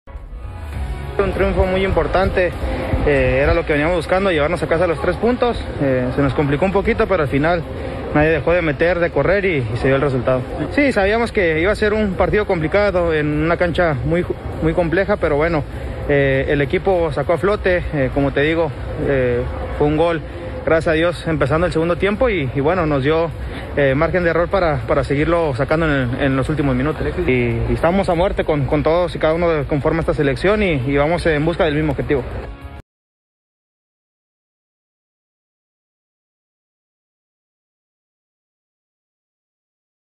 Jamaica vs México: Se nos complico un poquito pero al final se dio el resultado, Alexis Vega
El delantero Alexis Vega, fue el autor de uno de los goles que le dieron la milagrosa victoria a la selección mexicana.